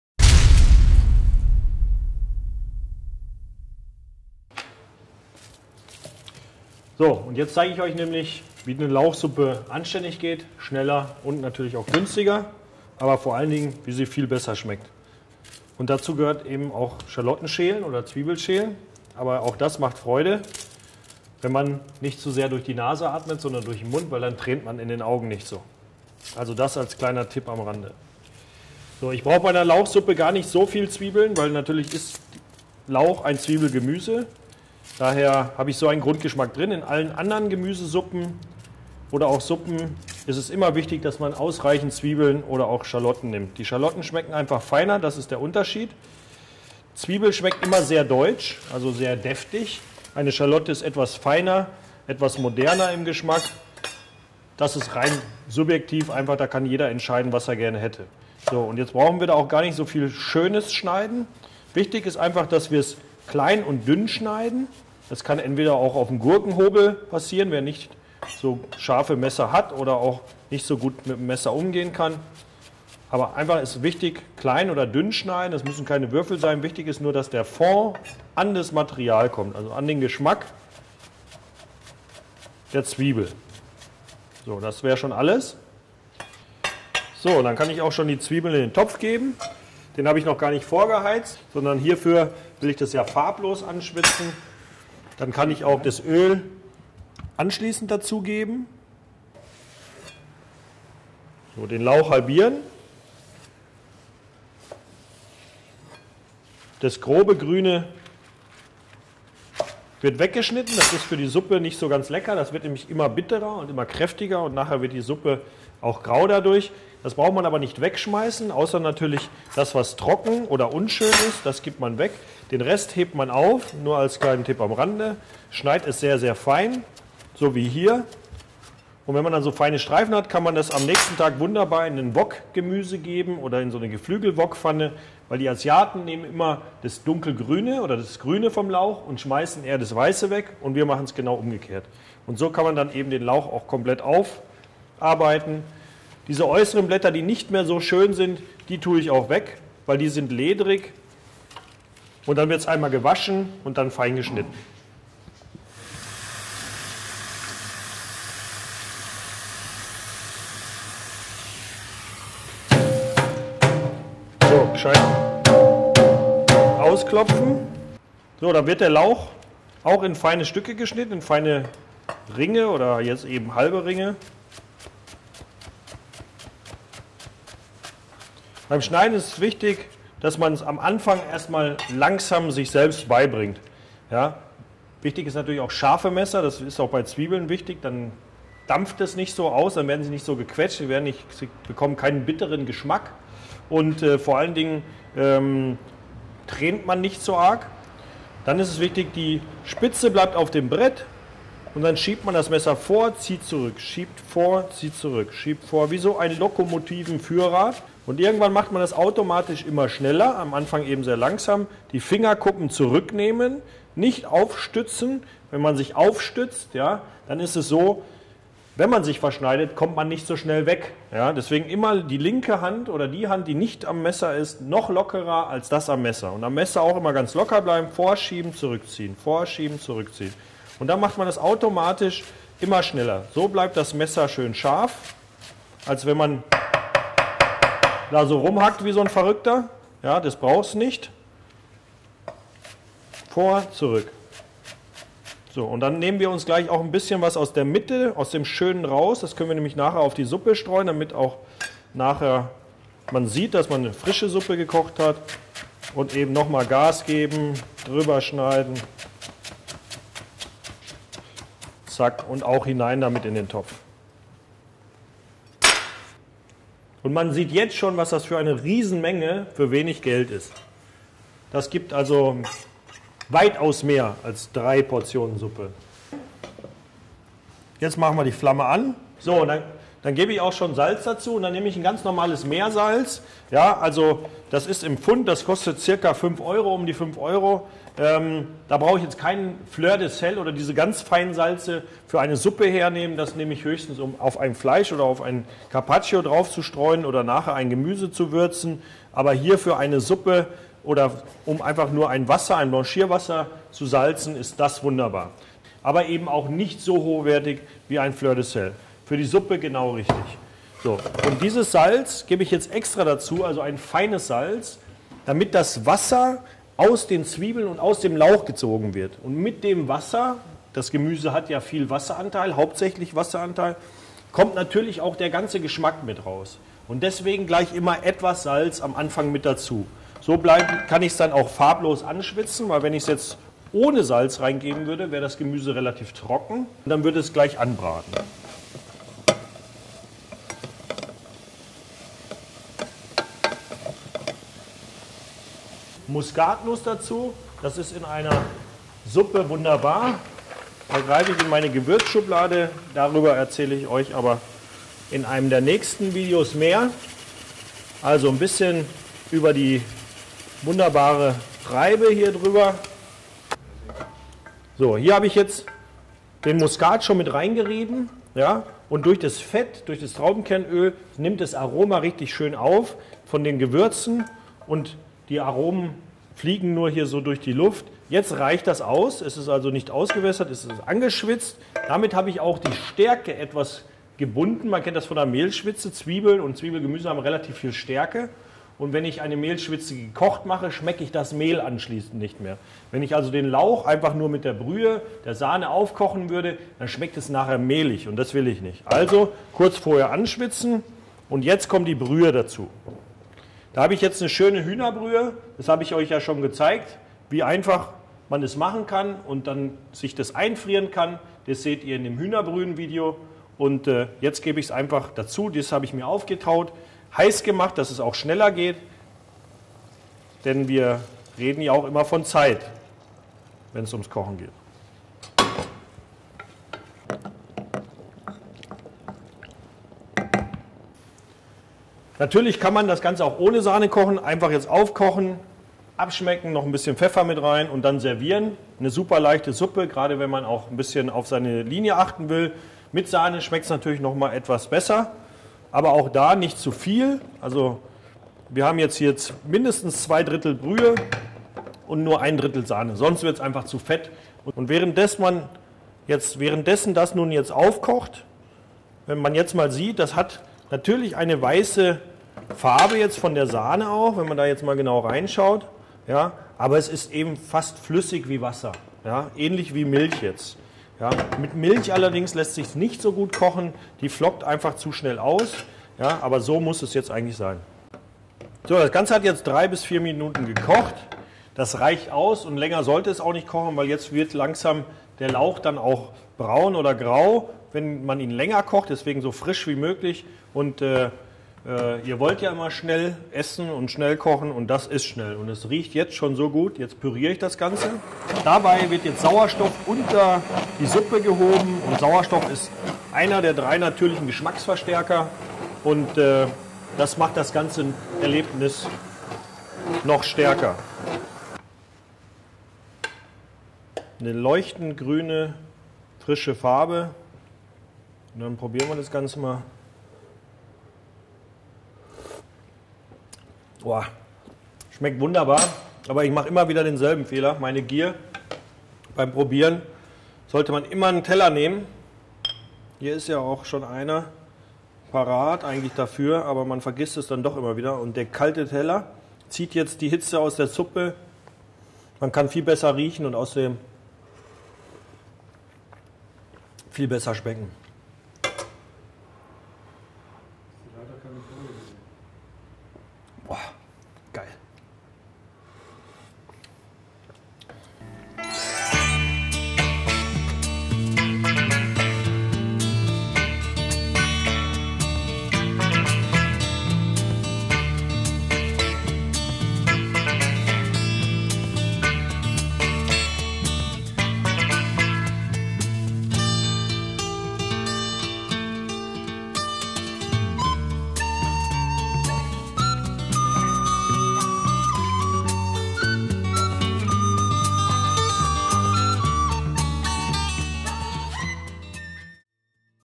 WM 2010, Holger Stromberg kocht Lauchsuppe
Die WM 2010 - mit dabei der Koch der Deutschen Nationalmannschaft, Holger Stromberg. Hier zeigt er wie es gemacht wird. Es gibt Lauchsuppe. Ein Kochrezept vom bekannten Sternekoch Holger Stromberg. Mehr von Holger demnächst hier. Es soll eine Serie entstehen. Dafür brauchen wir aber Eueren Input. Was wollt Ihr von Holger Stromberg wissen zum Thema Kochen?